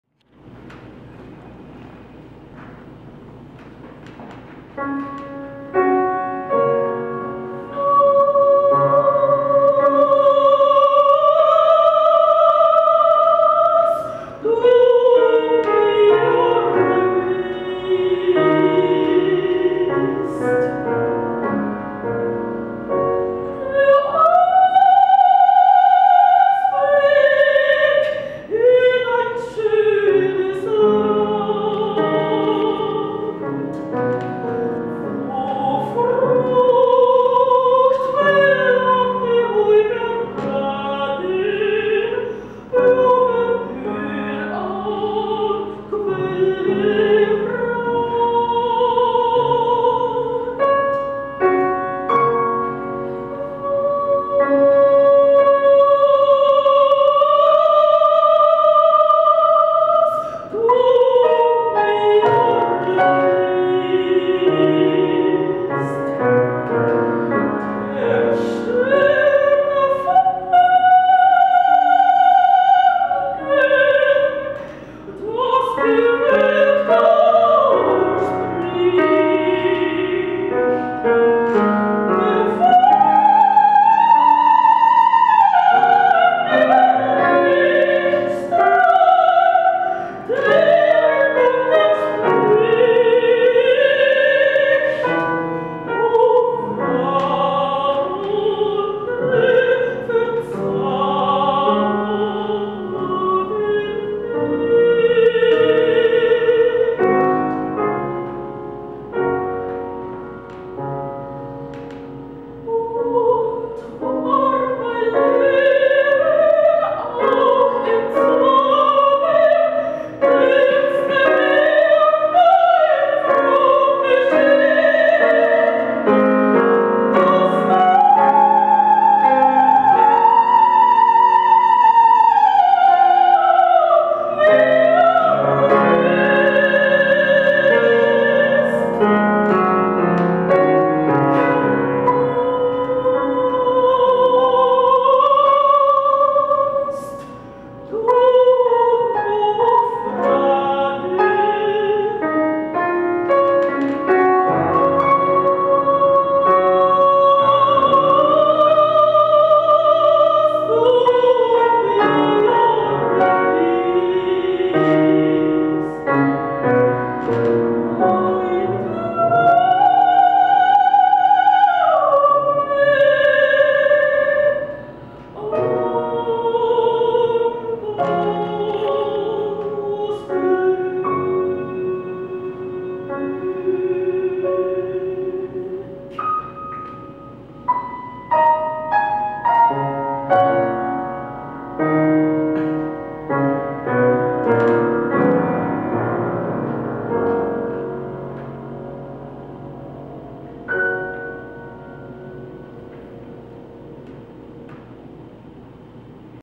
Betty Garces
Colombian Soprano Betty Garcés in concert at the Embassy of Colombia in Washington DC.